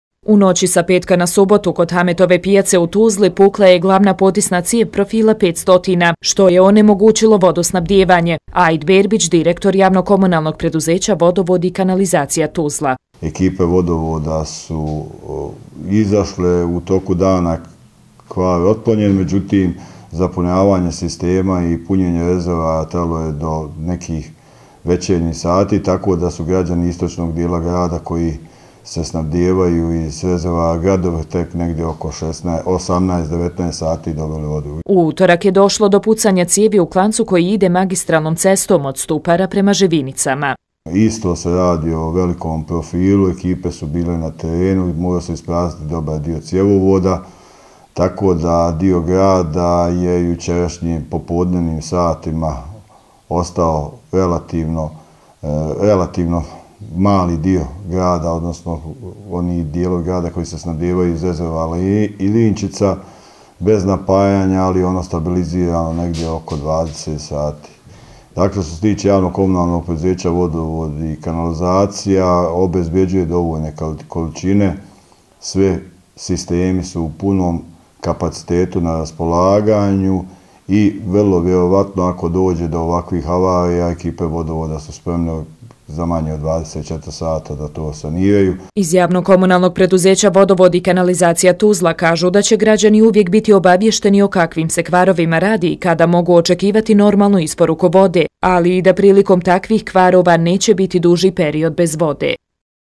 Veliki kvarovi i problemi u vodosnabdijevanju Tuzle rješavaju se u roku od 24 sata
Proteklih dana u Tuzli je dolazilo do većih kvarova zbog čega je bila onemogućena isporuka vode u pojedinim dijelovima Tuzle, ali i u ovakvim slučajevima kada su u pitanju veliki kvarovi, oni budu sanirani u roku od 24 sata. Trenutno je vodosnabdijevanje u svim gradskim i prigradskim naseljima uredno.